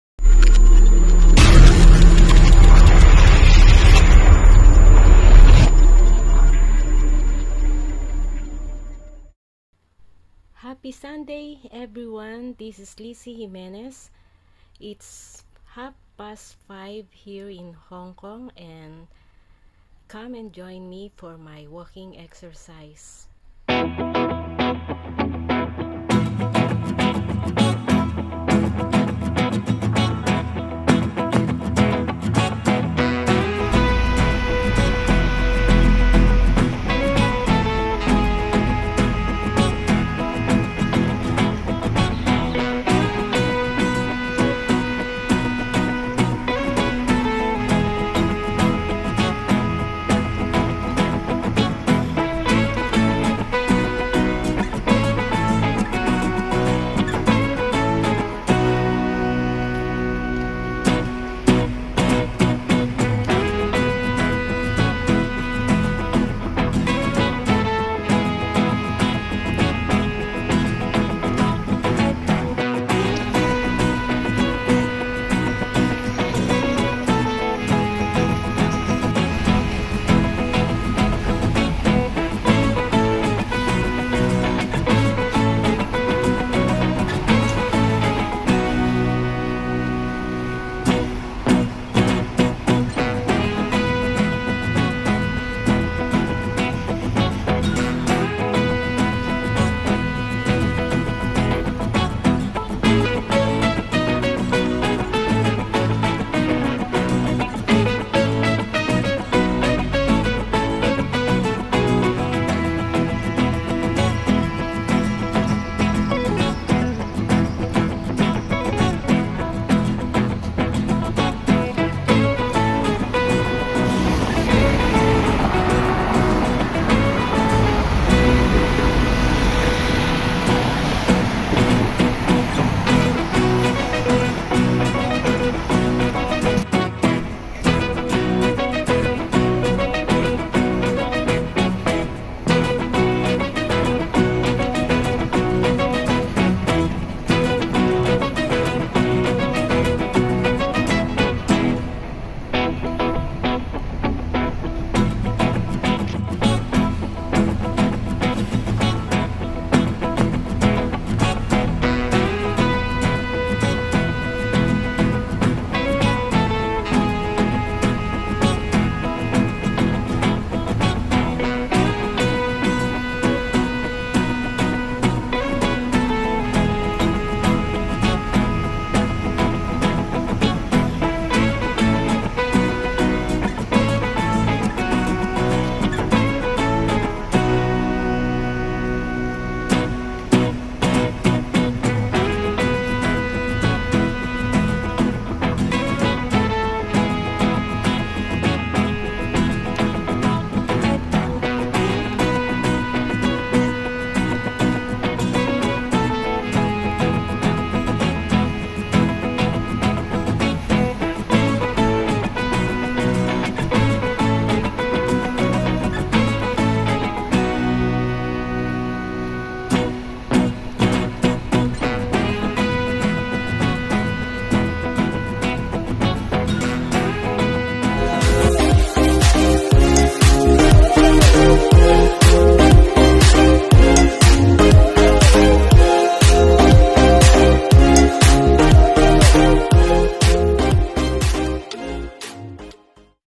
Walking Exercise, Sham Shuipo Hong Kong
WALKING EXERCISE
My Vlog Today

I get up early today to do my walking exercise around my area.

Come and join me, will take 30 mins walking today even the weather is not good this time.

Lizzie Jimenez

Music n this video provided by:
Kinemaster